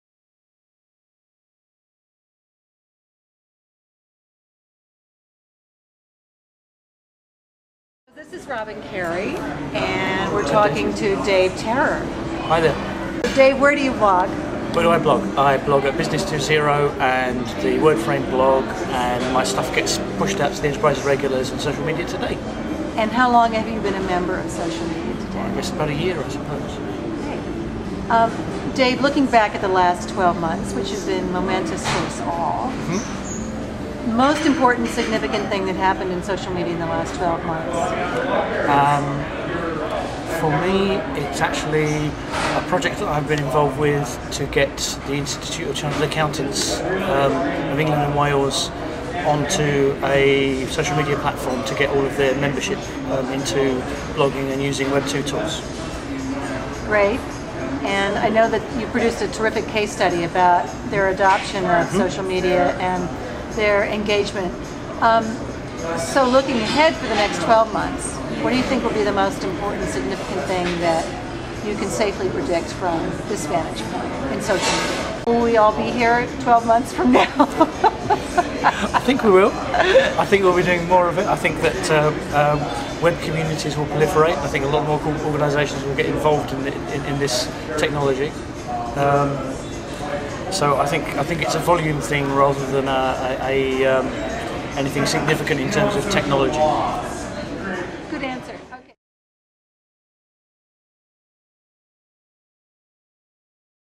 Social Media Today & David Terrar
As part of our September Birthday celebrations, we're asking our bloggers about the development of social media over the past year, and what we can expect in the future.